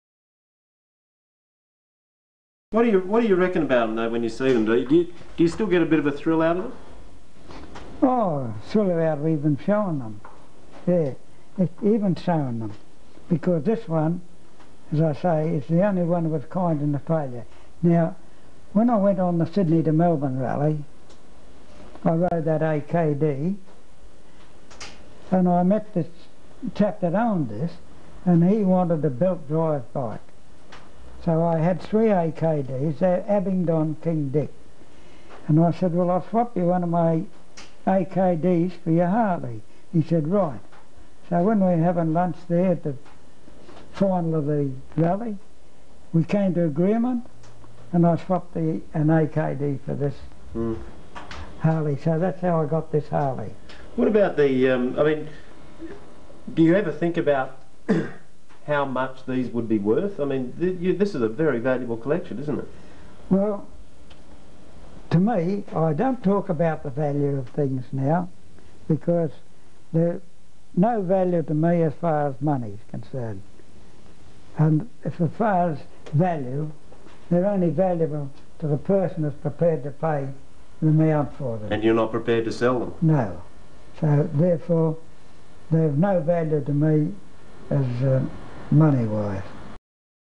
Harold Braund vintage motorcycles 5
A story on a 1913 Harley Davidson singe chain-drive with back pedel brake he swap it for a 1912 AKD single belt-drive